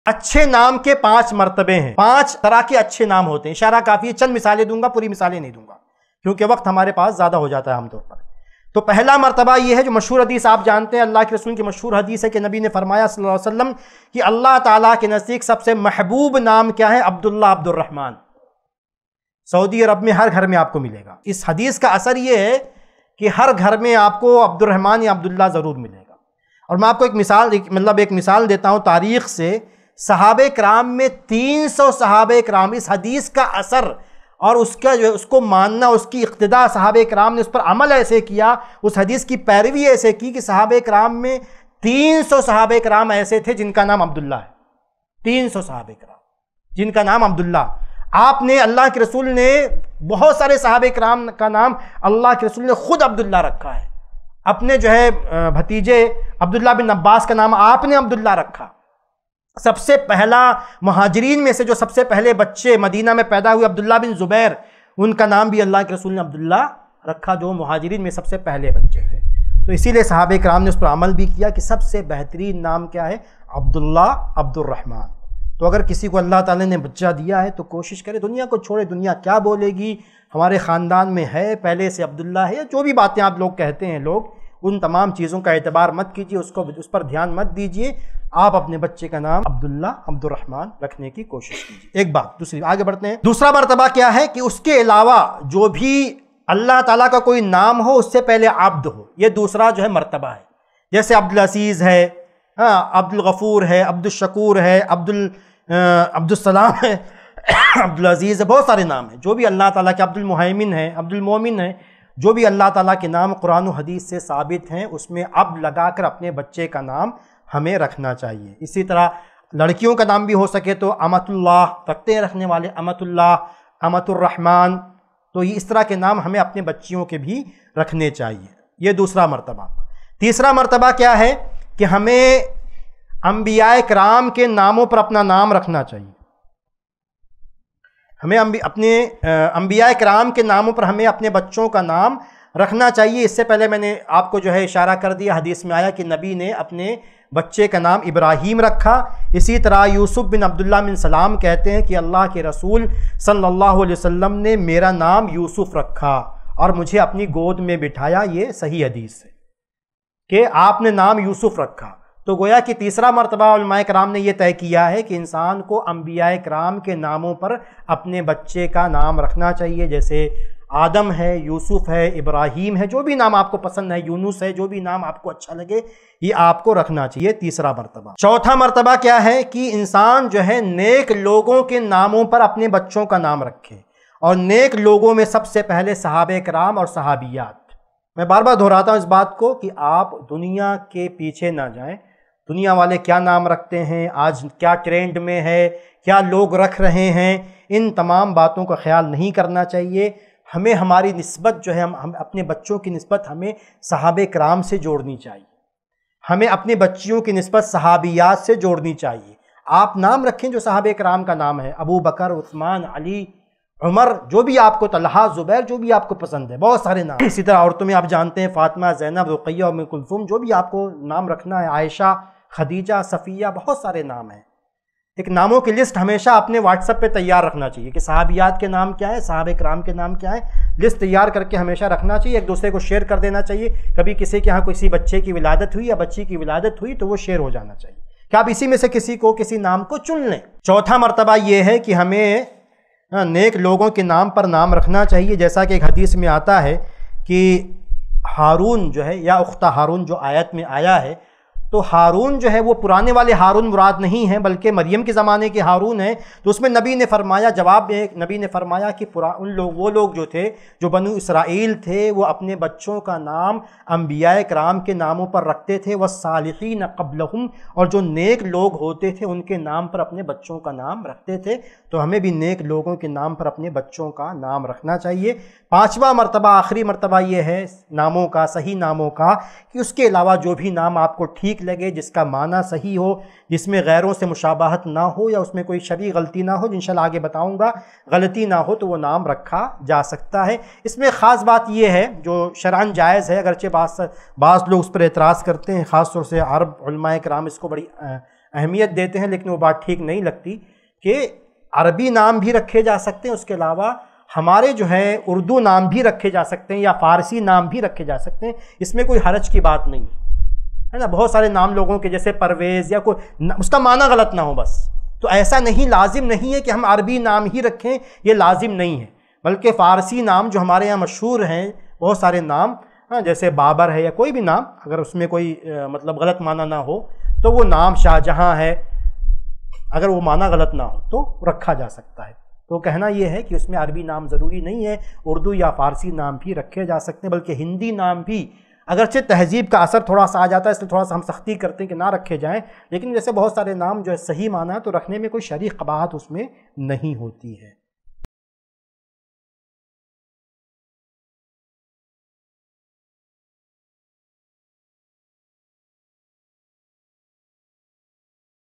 Bachche Ke Naam Rakhne Ke Yeh 5 Martabe Hote Hain By Shaikh Abdus Shakoor Madani
Bank Account Details:
ILM FOUNDATION
ICICI Bank
Savings account
IFSC CODE-ICIC0001028